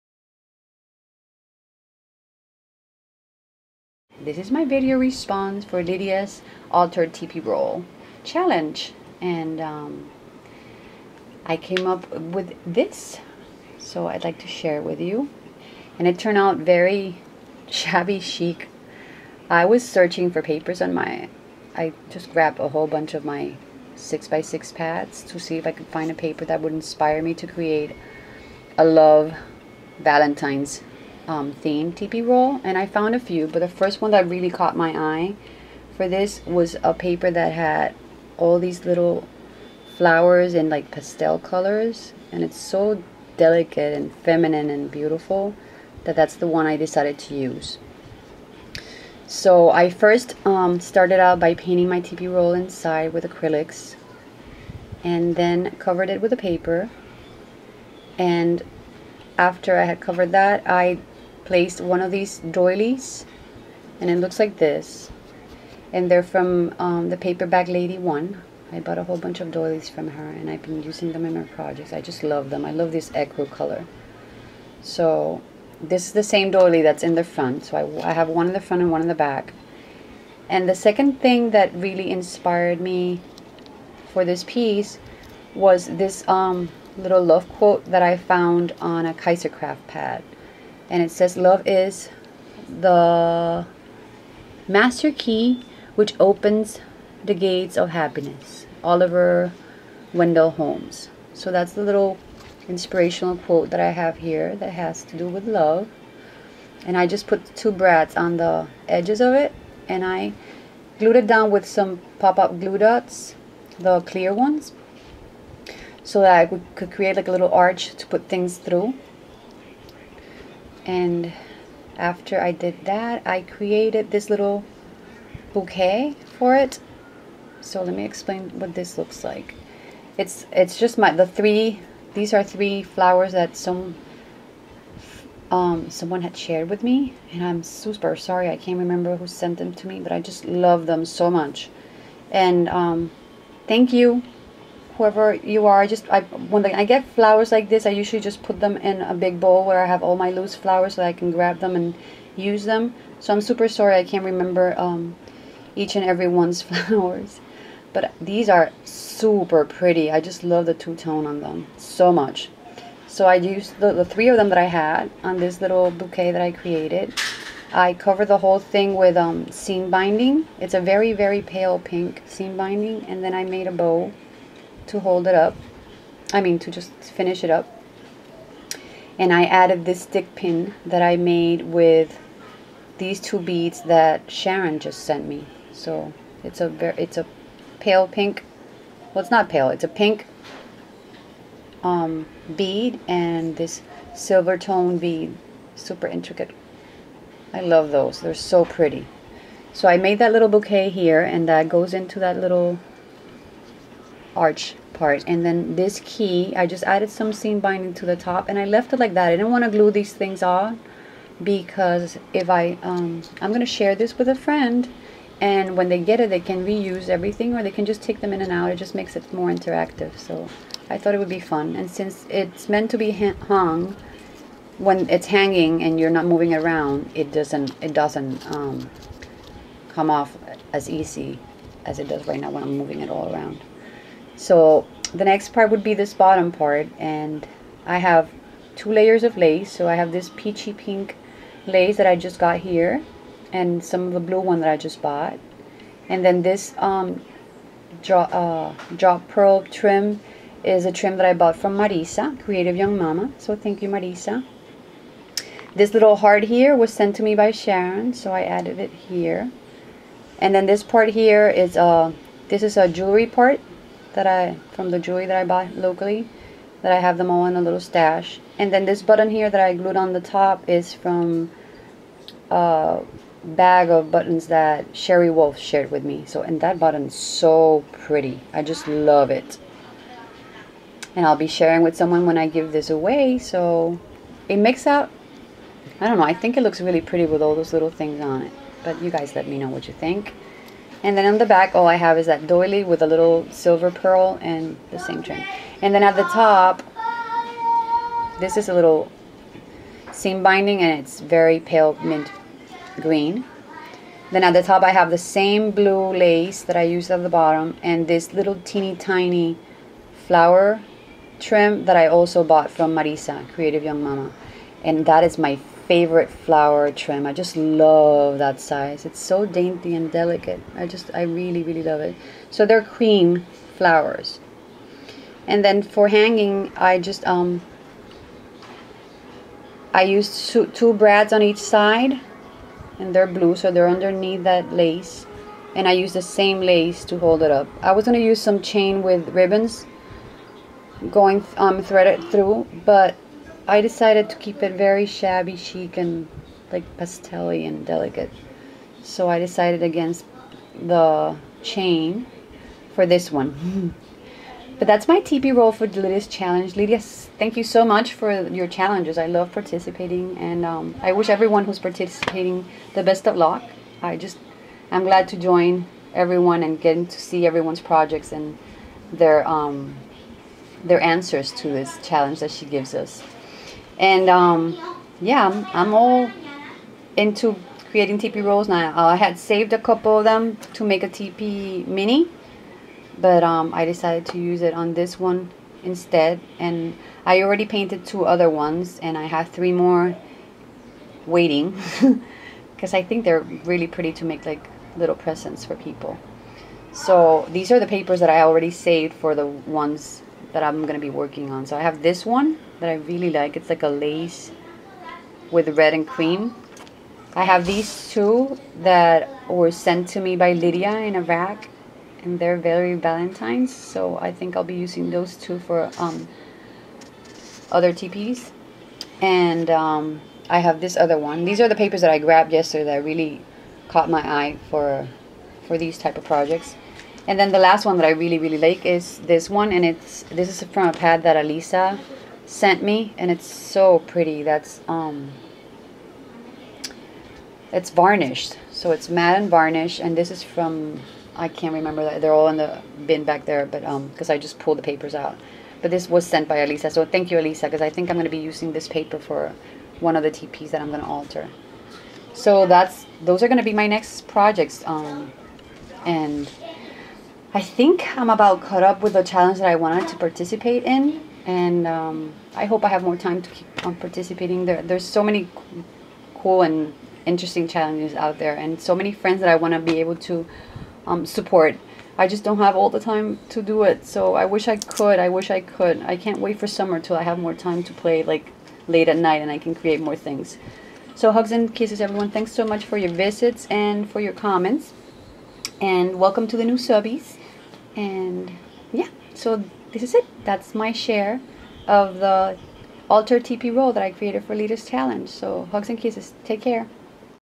VR for ItripledogdareU altered TP Challenge
Hello my friends this is my altered TP for Lydia's Love or Valentine's theme Challenge. I hope you all get a chance to visit her channel and join in the fun. TFW, hugs, Evy